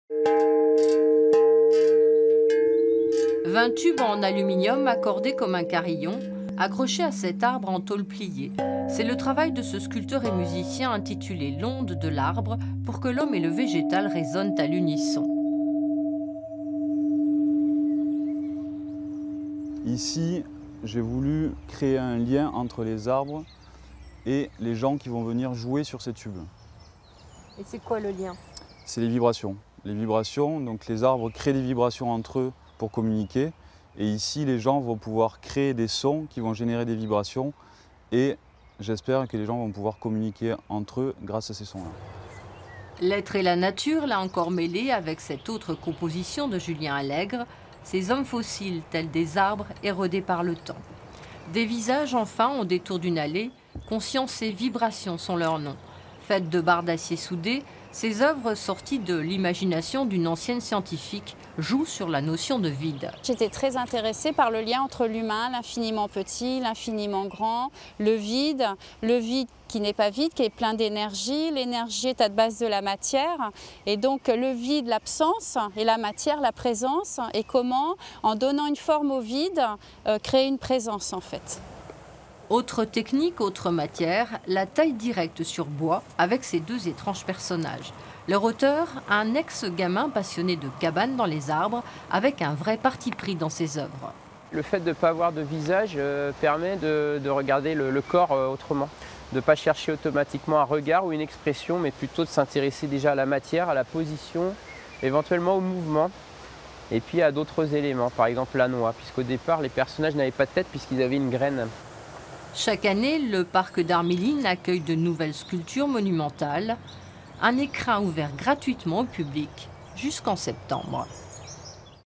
Châteaubourg : le jardin des arts, une galerie à ciel ouvert
Et si vous alliez faire un tour au jardin ? Direction Châteaubourg et son parc Ar Millin où depuis 2003 l’association "Les Entrepreneurs Mécènes" propose au public de se frotter à l’art monumental.

Un reportage de C. Jauneau, T. Bouilly, T. Descamps / avec Julien Allègre, sculpteur - Isabelle Garbil Fauve-Piot, sculptrice - Nicolas Vasili Barbé, sculpteur